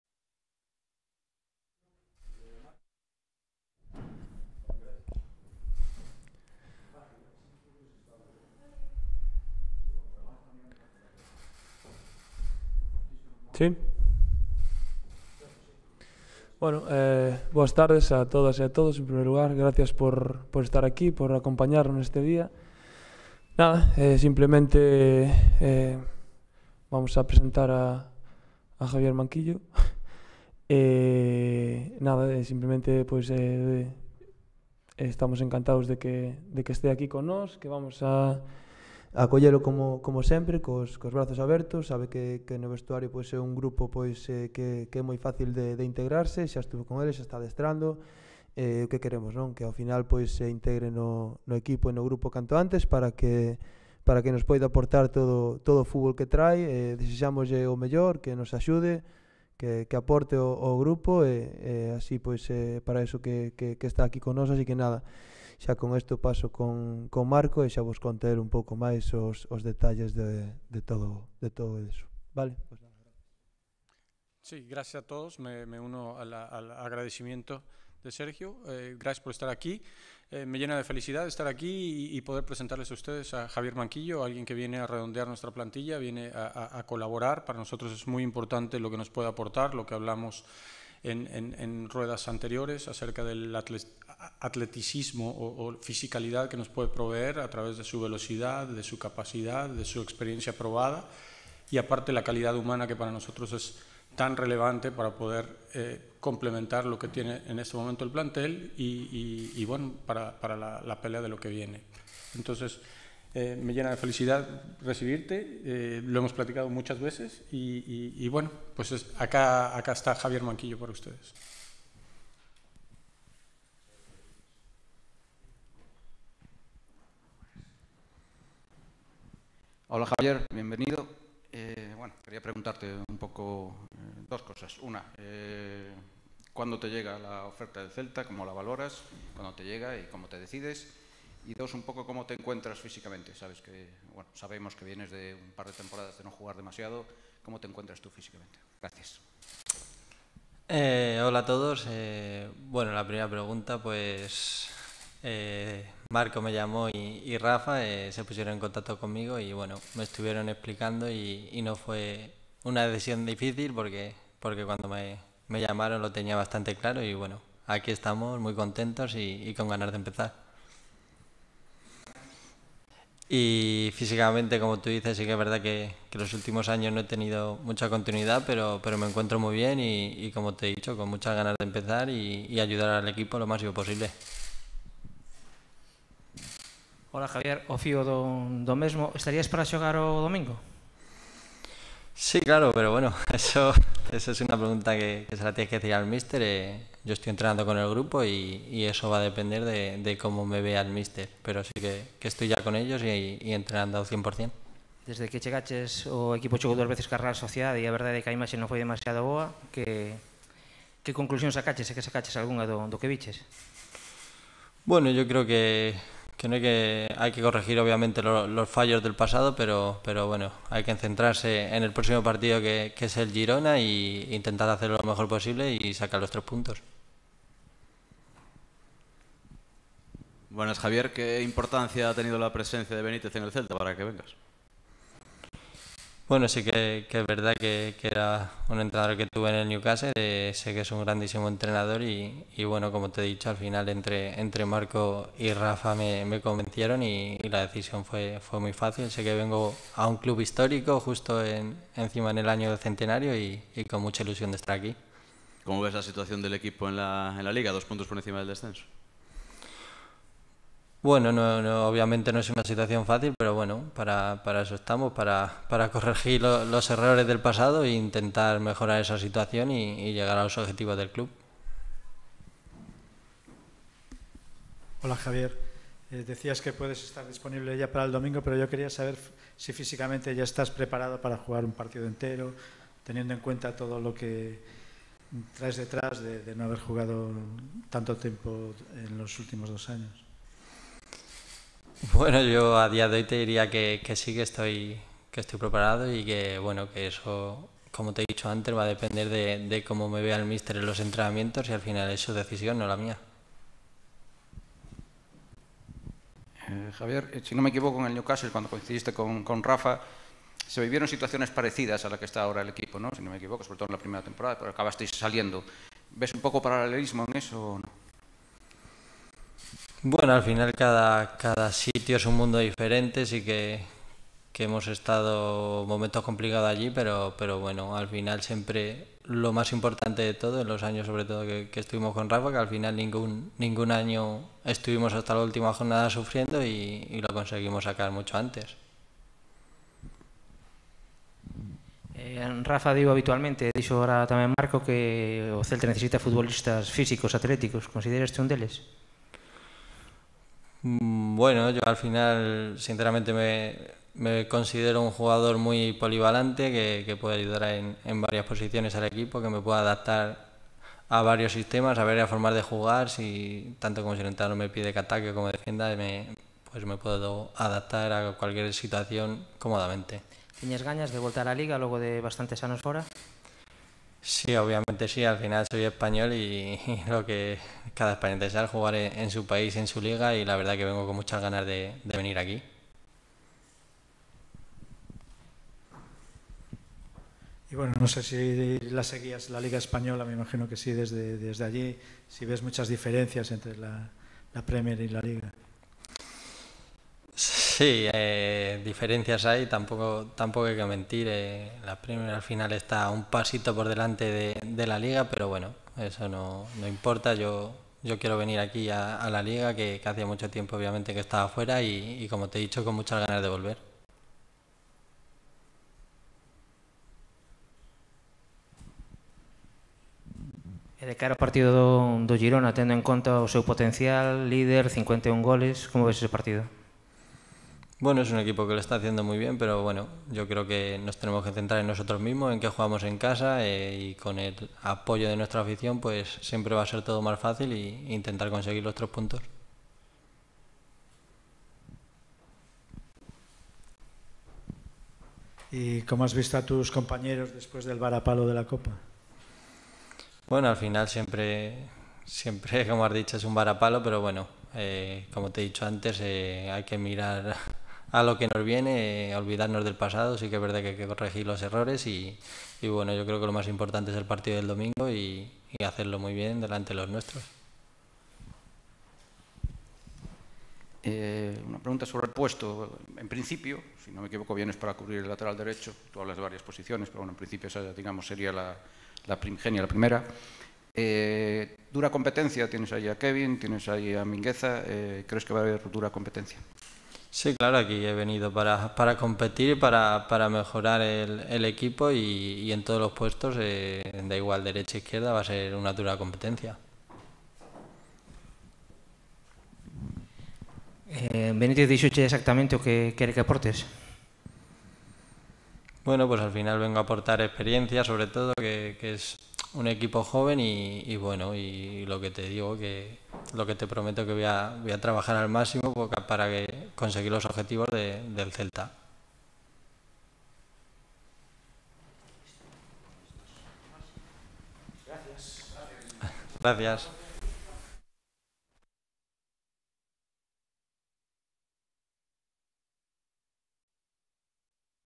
Presentación oficial de Javier Manquillo como nuevo jugador del RC Celta
Rueda de prensa de la presentación oficial de Javier Manquillo como el último fichaje del RC Celta.